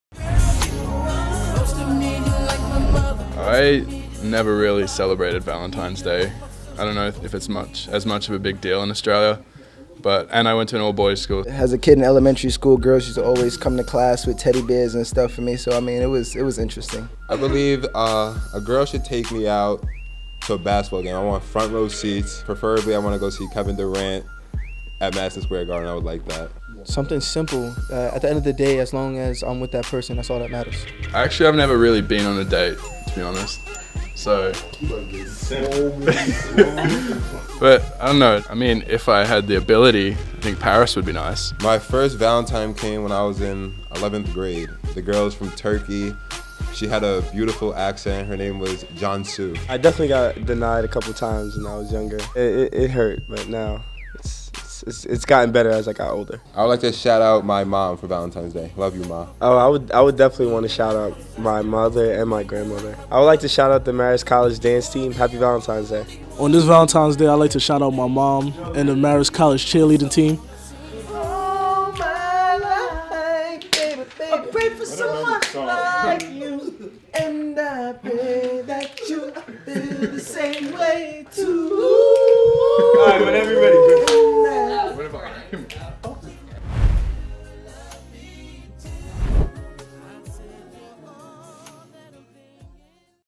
Marist Men's Basketball: Romance with the Foxes ❤
Ever wonder how to give a Red Fox the perfect Valentine?? (Hint: It involves court side seats).